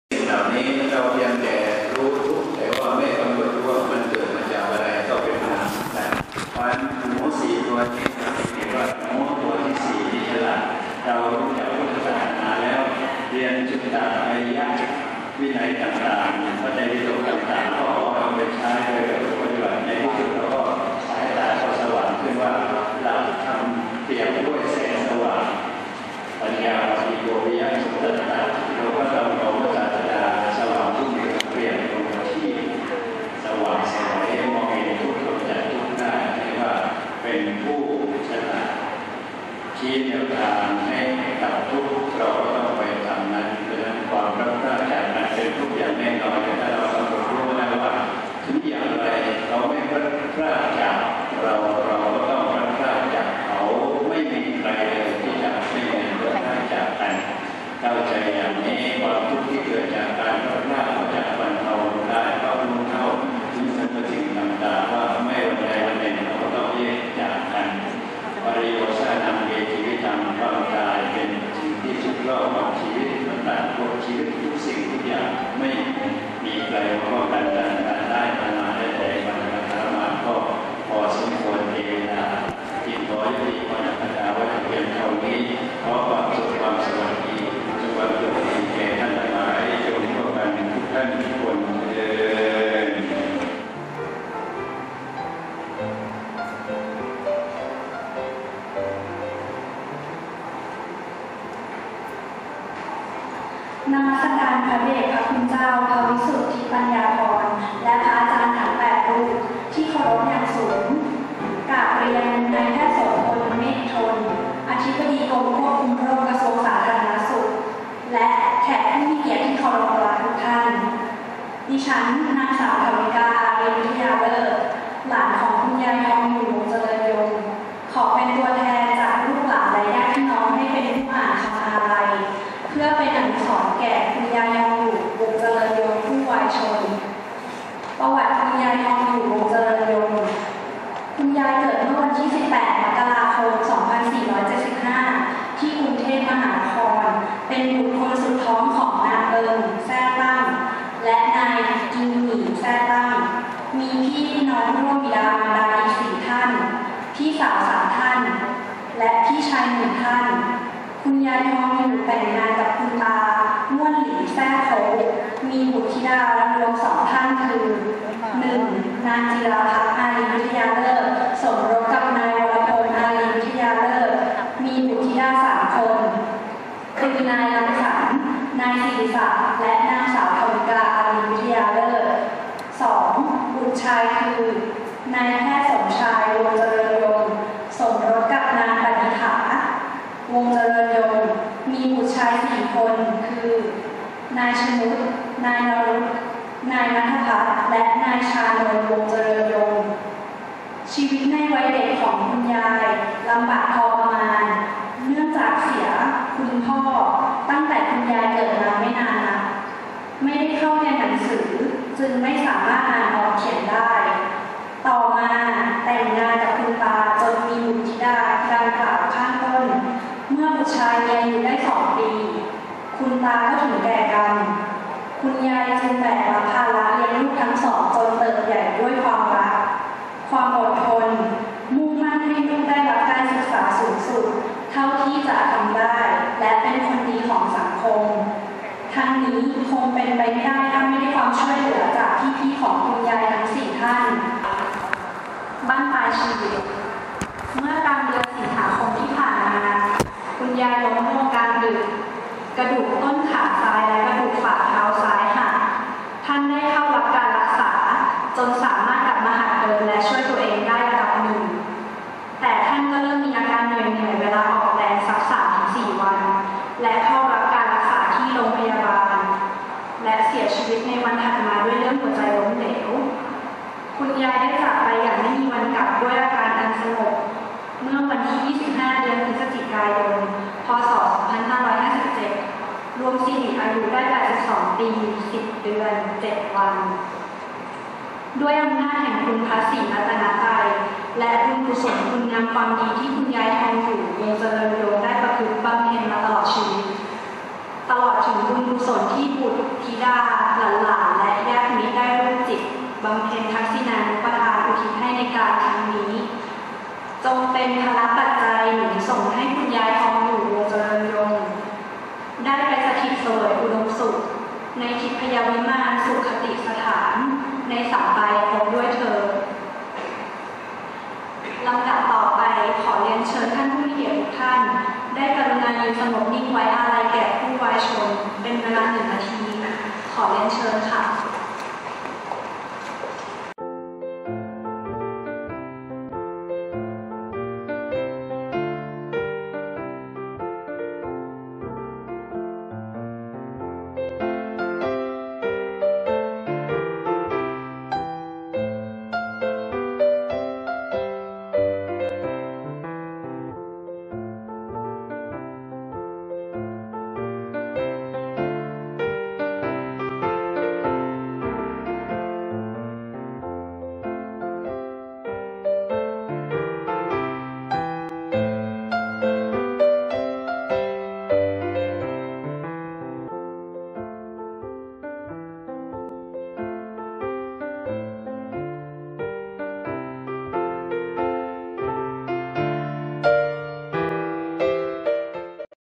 funeral Doctor Somchai Mom
งานฌาปนกิจศพ นางทองอยู่ วงศ์เจริญยง
ณ วัดชลประทานรังสฤษดิ์ จ.นนทบุรี
วันอาทิตย์ที่ ๓๐ พฤศจิกาบน ๒๕๕๗